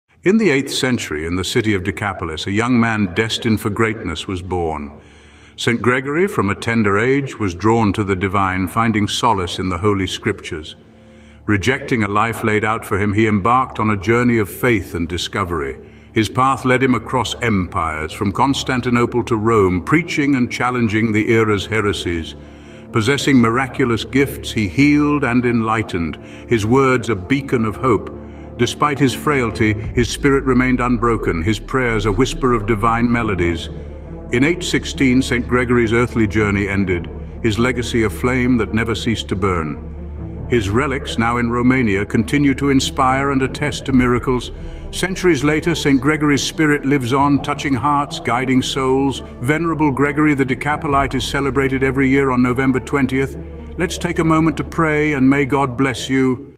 Saint Venerable Gregory the Decapolite - November 20th
Join us at 'Orthodox Saint of the Day' as we honor Venerable Gregory the Decapolite, honored on November 20th. Journey with us into the life of this devout 8th-century ascetic, whose story is woven into the fabric of early Christian history, marked by the rise of the Iconoclast controversy and the spiritual challenges of his era.

Discover Saint Gregory's early years in the city of Decapolis, where his love for the Church and the Holy Scriptures set him on a path of unwavering faith. Learn about his bold decision to leave home and his remarkable travels across Constantinople, Rome, and Corinth, preaching the Word of God and strengthening the Orthodox faith.

Witness the extraordinary spiritual gifts of Venerable Gregory, from his healing abilities to his profound prophecies. His devotion and miraculous works amidst persecution illuminate the strength and resilience of those who fervently uphold their beliefs.

Follow Saint Gregory's journey from a wandering ascetic to a revered saint, leaving an indelible impact on the Christian world. His relics, now resting in Romania, continue to inspire faith and devotion, bridging the past and present.

Delve into his spiritual legacy and the enduring influence of his holy life that echoes through time.